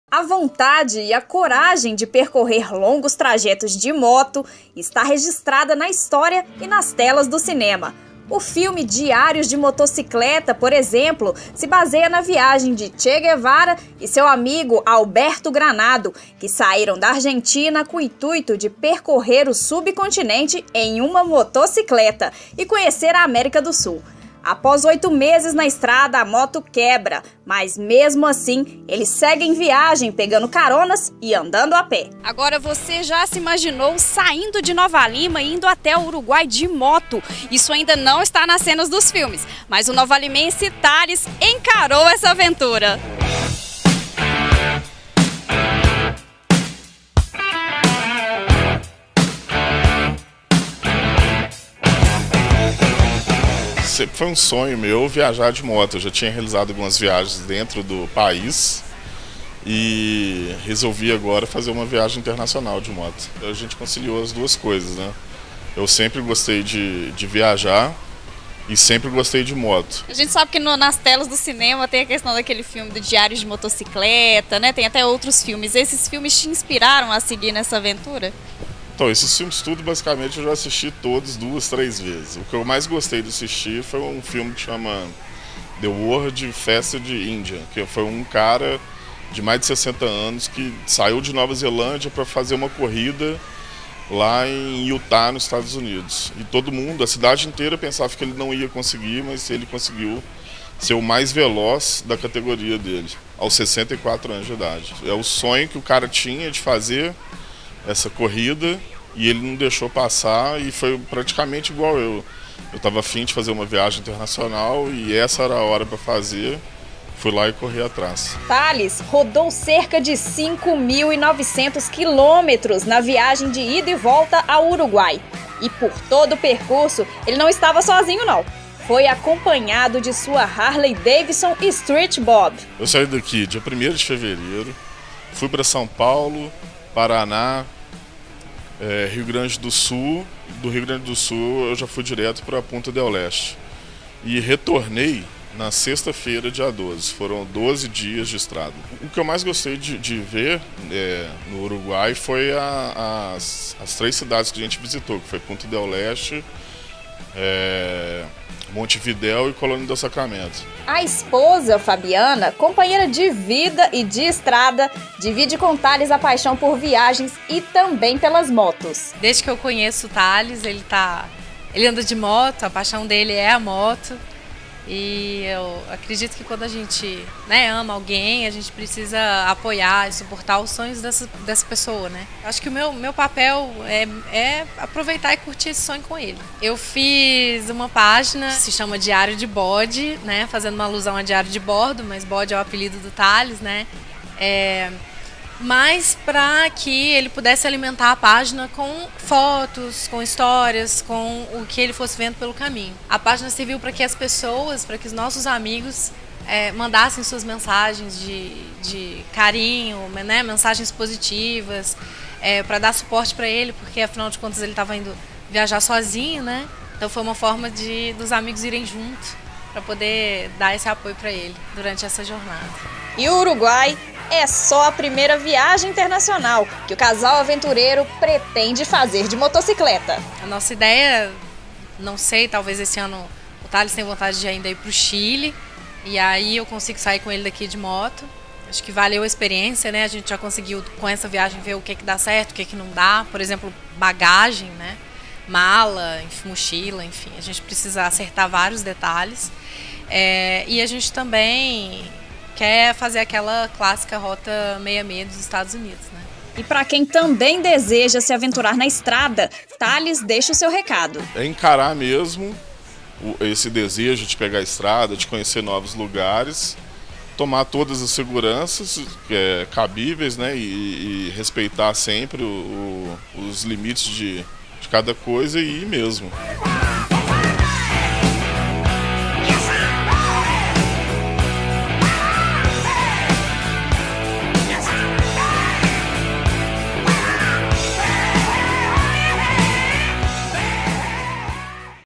TV Banqueta - Jornal Banqueta - Viagem de Nova Lima ao Uruguai de motocicleta - 17/02/2016
O engenheiro nova-limense, Thales Simon, viaja de Nova Lima ao Uruguai em cima de um motocicleta.